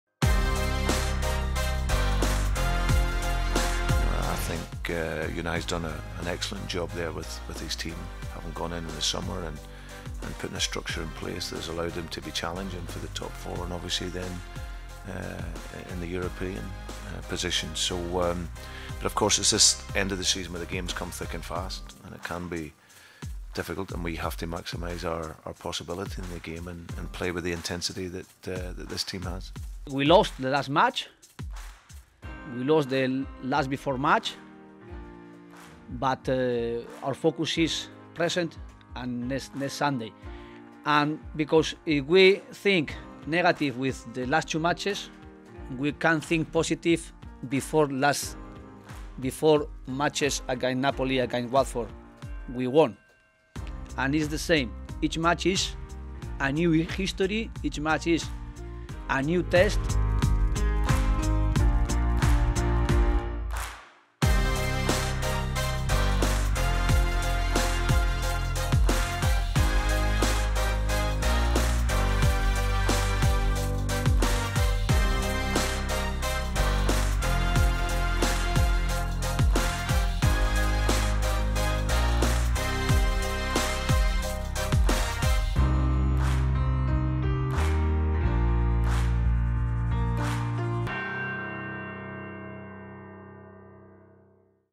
Leicester v Arsenal - Premier League Match Preview
An in-depth look at Arsenal's Premier League trip to Leicester.